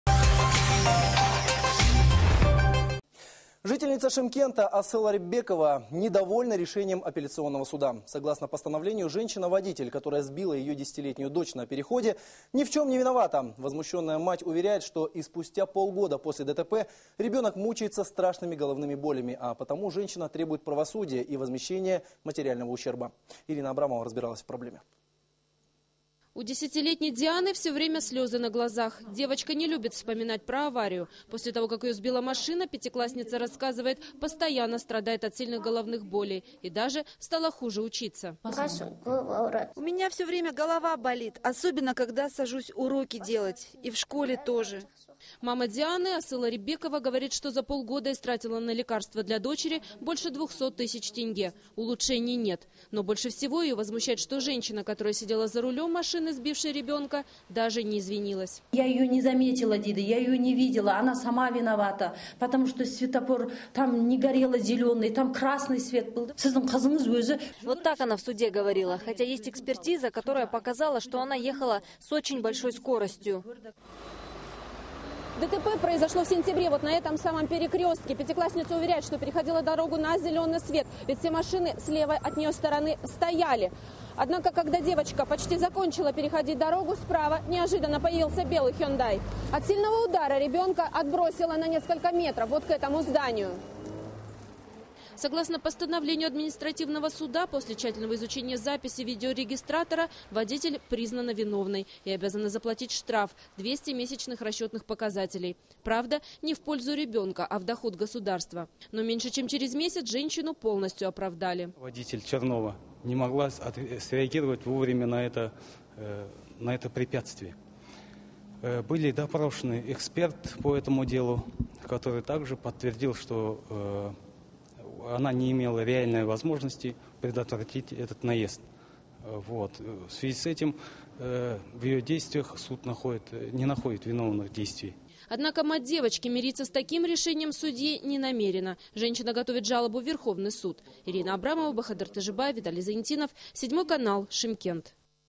Водителя, сбившего ребенка, освободили от ответственности
Жительница Шымкента Асыл Арипбекова недовольна решением апелляционного суда. Согласно постановлению, женщина-водитель, которая сбила ее 10-летнюю дочь на переходе, ни в чем не виновата.